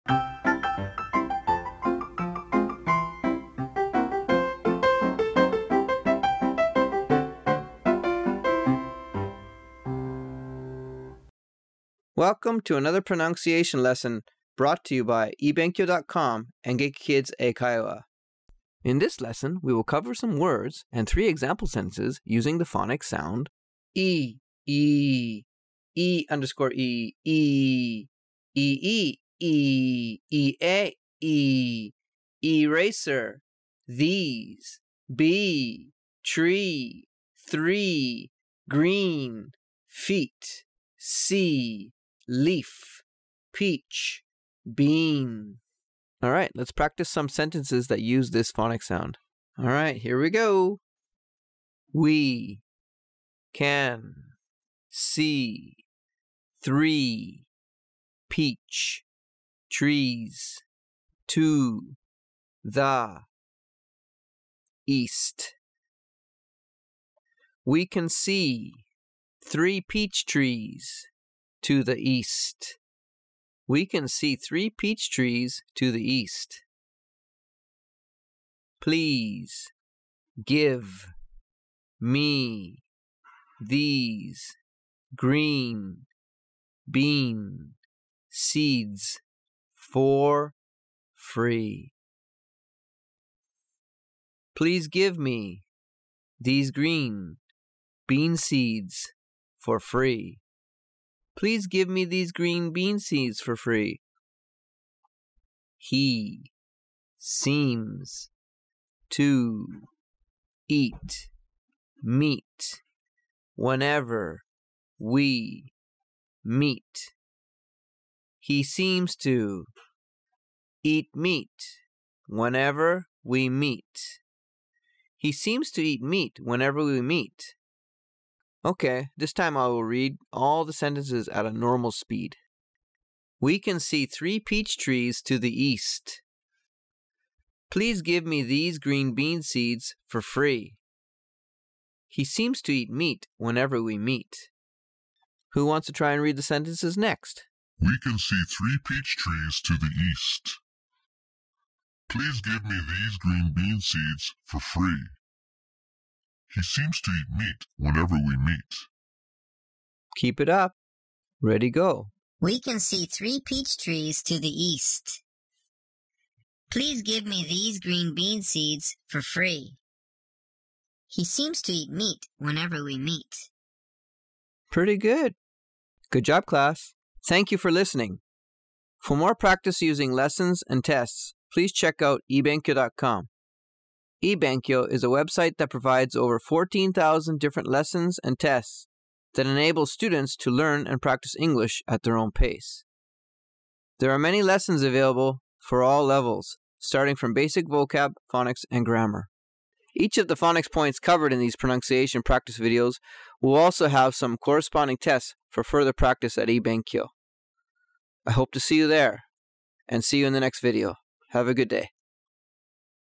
Phonics Lesson #141 Long E Ee Ea Sentences フォニックスレッスン - Long E Ee Eaの例文
In this video we will practice 3 example sentences and some vocab for the phonics sound Long E Ee Ea.
このビデオではLong E Ee Eaの例文と単語の練習します。

For more practice with this lesson or other lessons and tests, please

もっと練習したかったら、 へ行けば14000以上のレッスンとテストを利用して楽しく学べます。